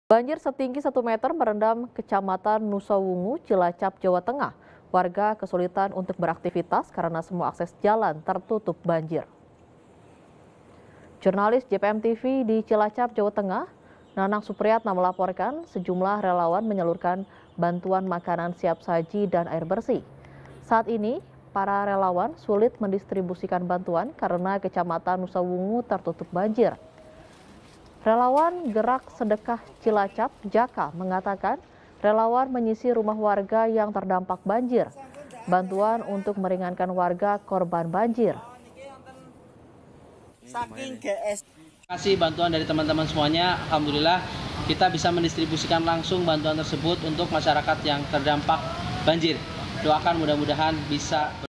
Relawan Sulit Menembus Lokasi Banjir di Kecamatan Nusawungu
Banjir setinggi satu meter merendam kecamatan Nusawungu, Cilacap, Jawa Tengah. Warga kesulitan beraktivitas, karena semua akses jalan tertutup banjir.